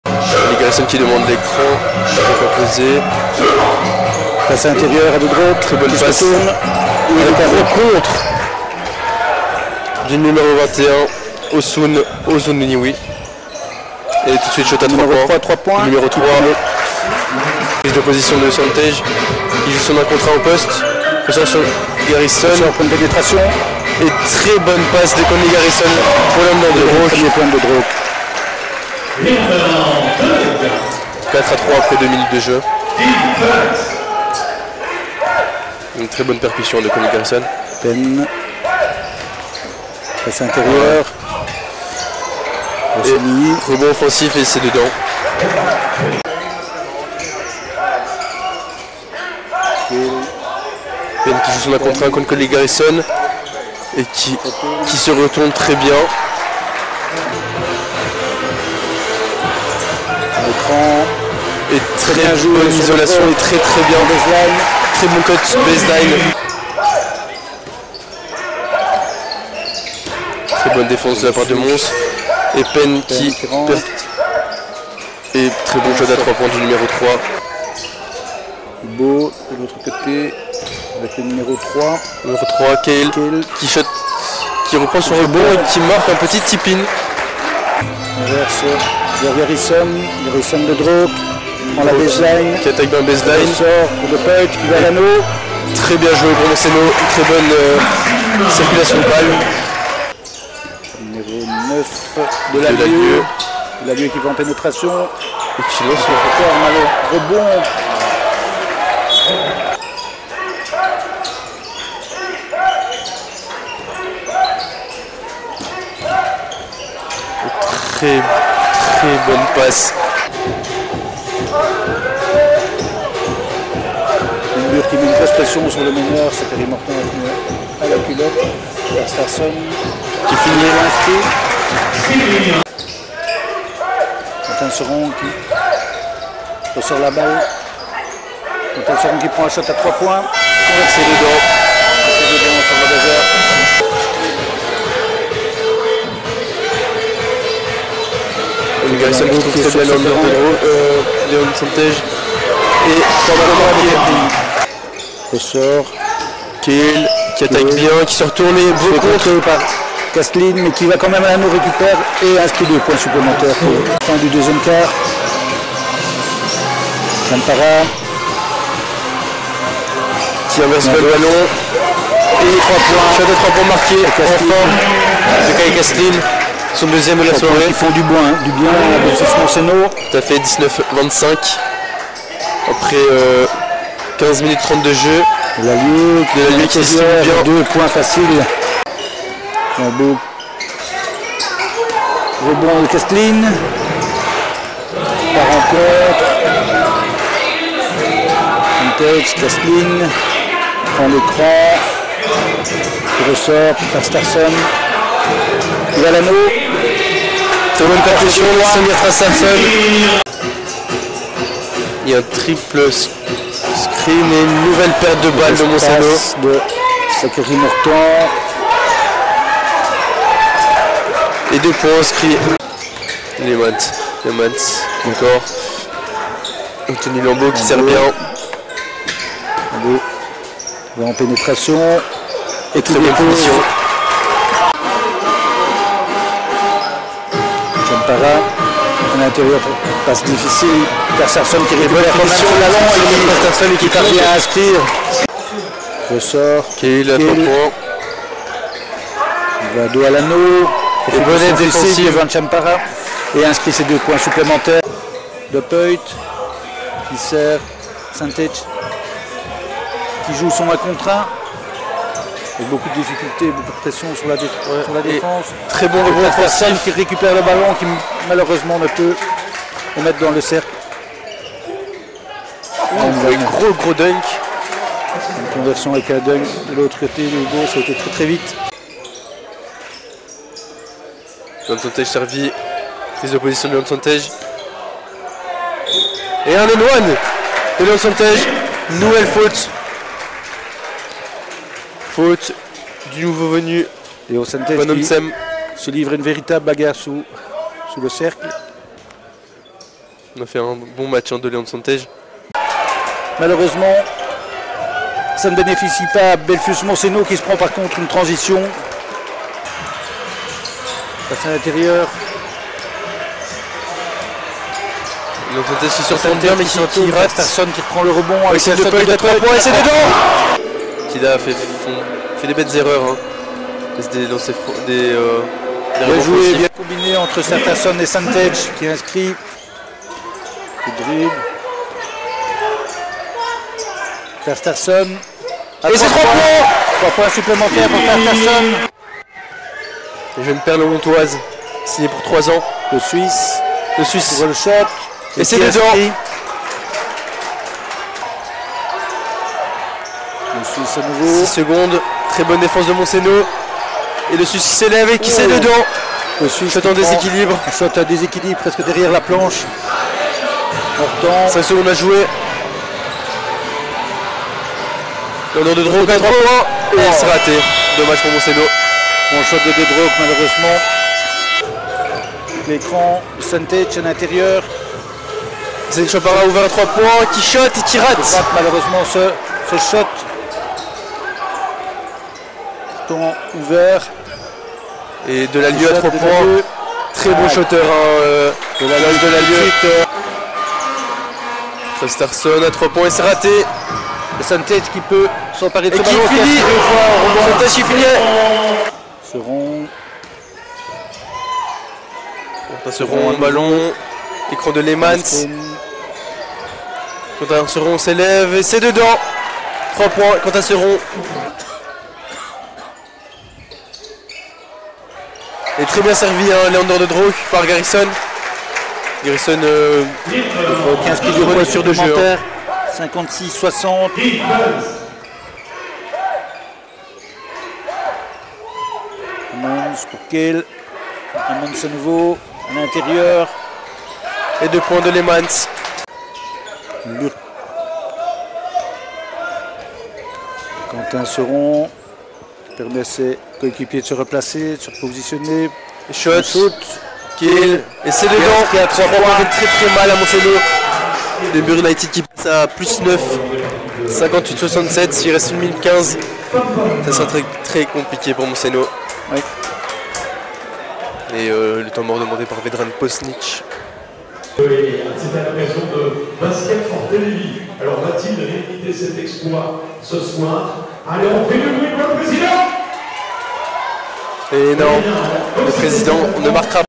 Belfius Mons-Hainaut vs. Hubo Limburg United - Game Highlights
Watch the Game Highlights from Belfius Mons-Hainaut vs. Hubo Limburg United, 12/02/2023